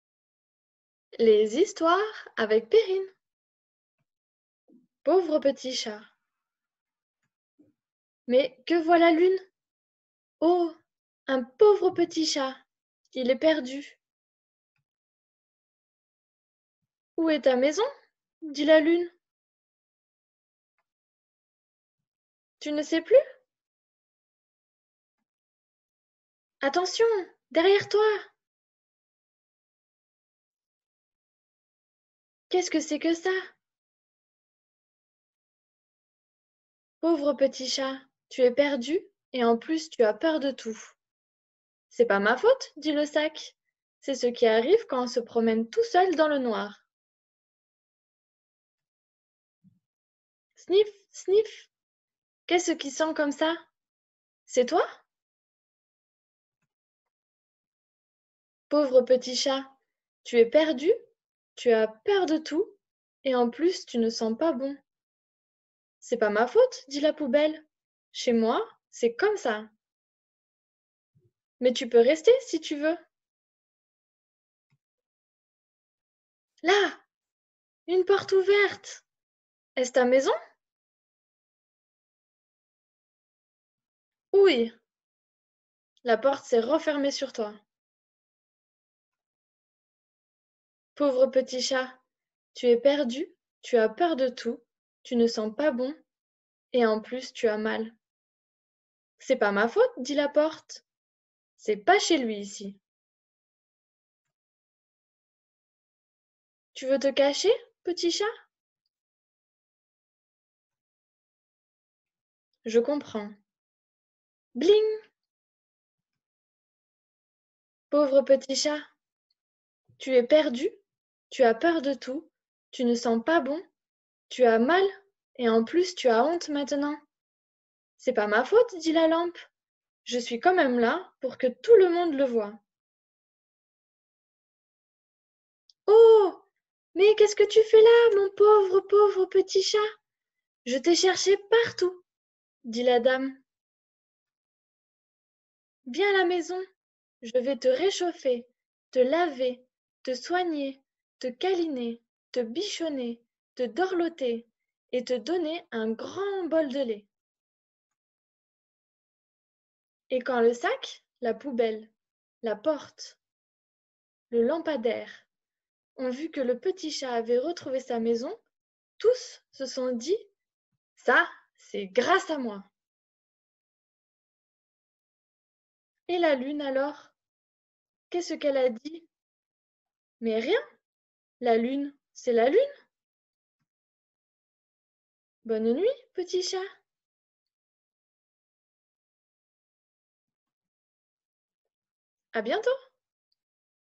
L'HISTOIRE du "PAUVRE PETIT CHAT"
Je vais vous raconter l'histoire d'un pauvre petit chat 🐈

Le lien de mon compte Instagram